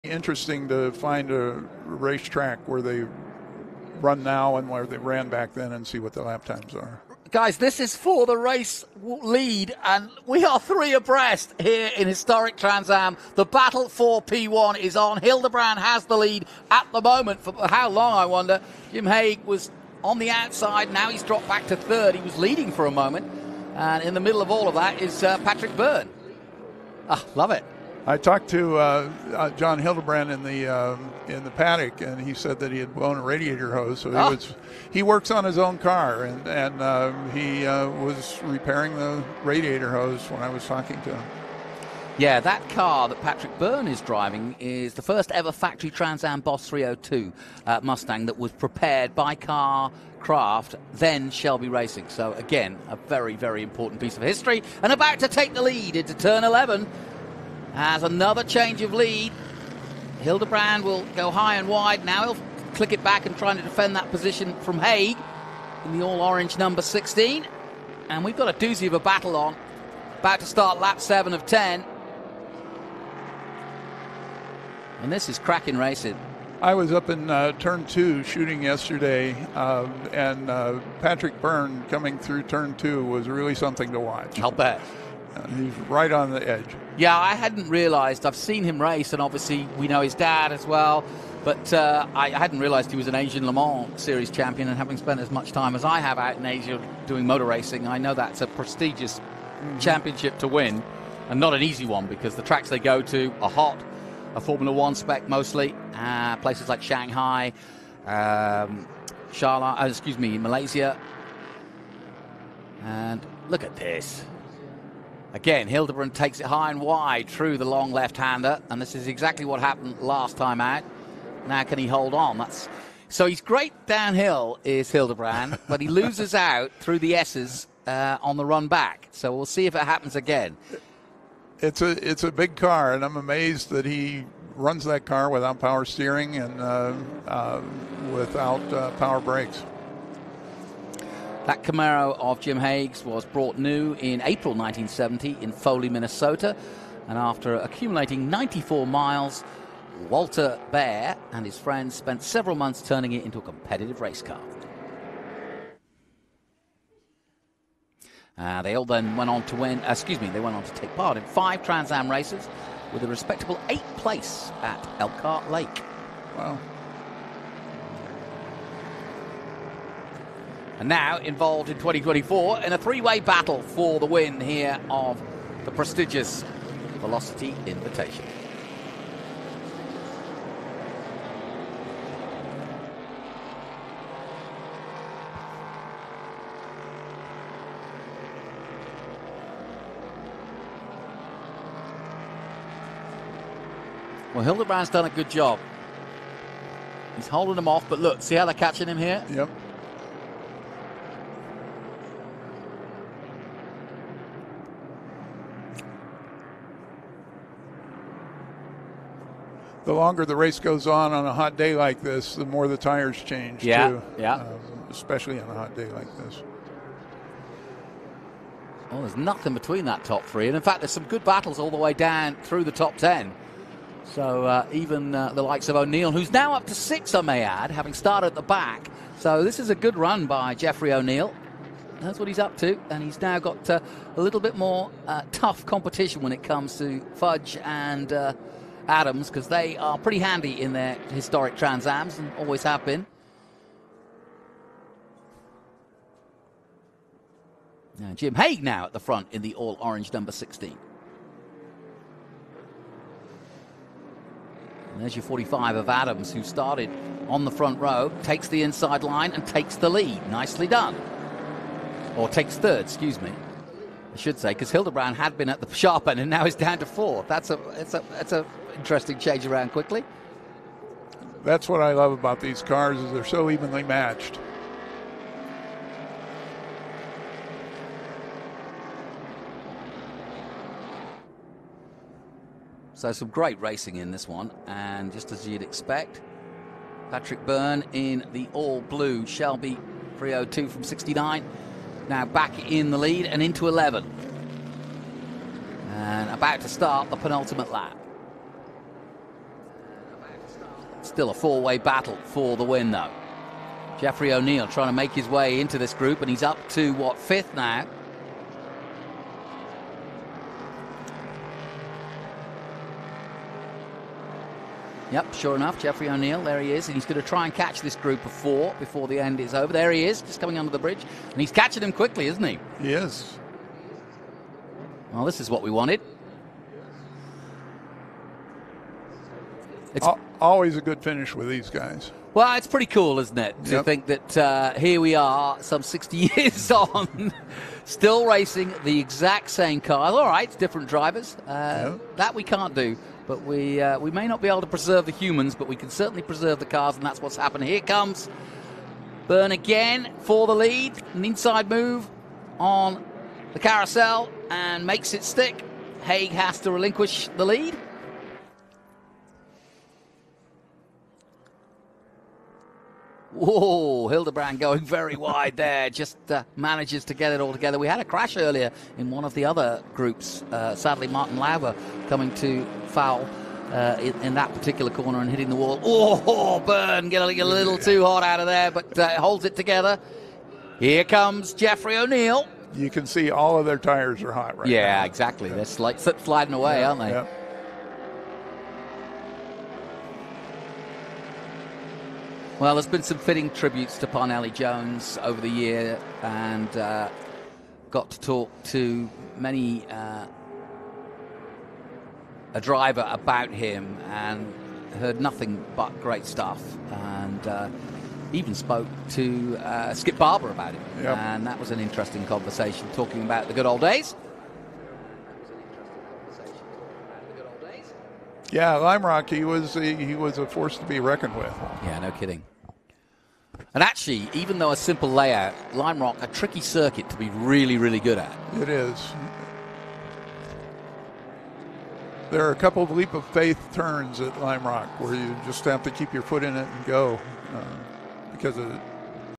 WHAT A FINISH! An incredible 4-way battle for the win in the Historic Trans-Am race
They were magnificent to watch in period, and they're just as magnificent to watch today. The Historic Trans-Am cars did not let anyone down when it came to providing some proper racing action at Sonoma Raceway. Even the 1964 Pontiac GTO got involved in the action.